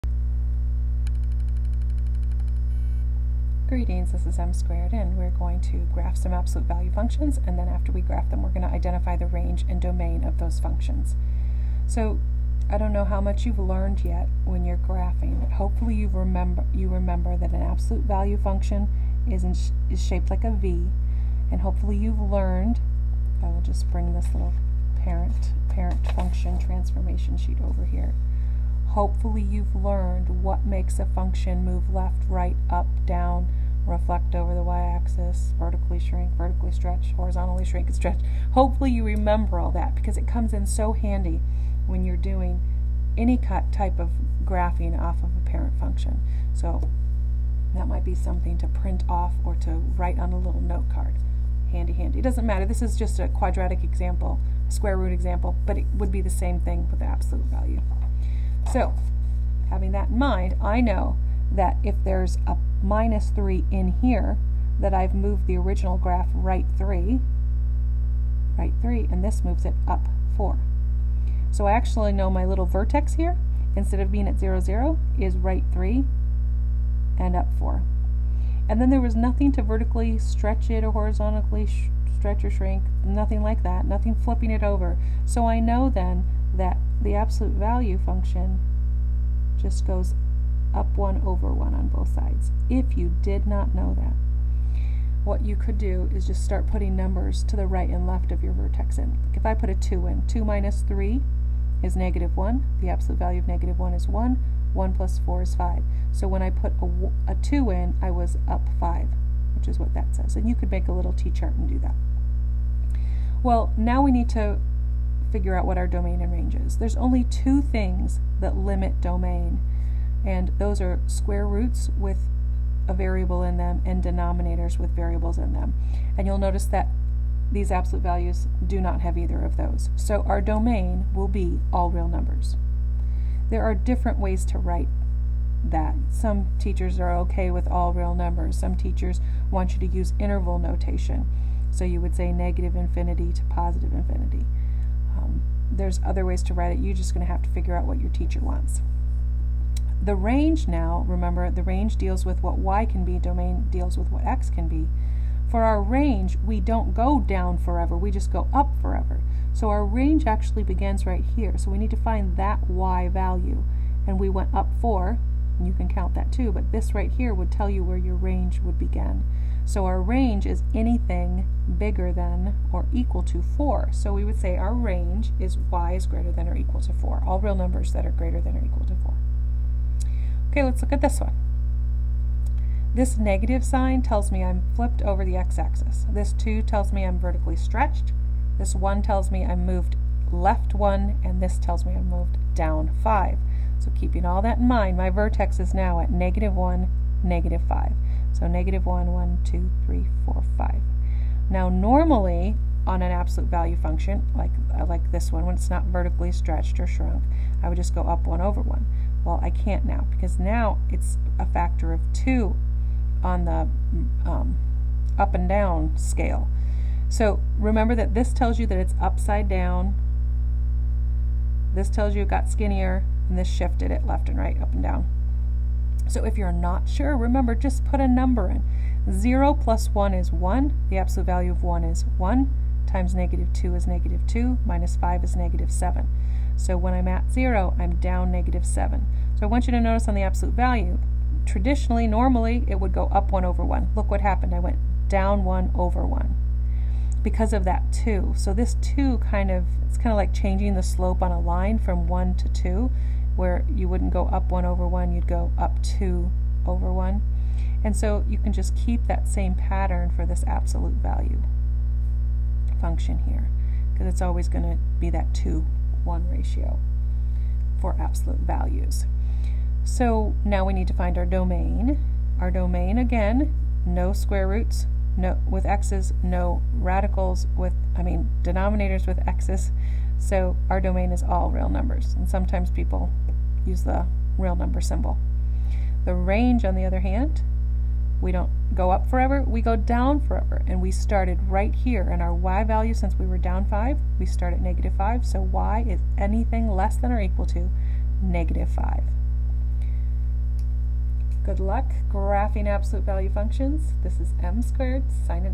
Graphing Absolute Value Functions & finding range and domain.mov
Graphing absolute value functions and finding the range and domain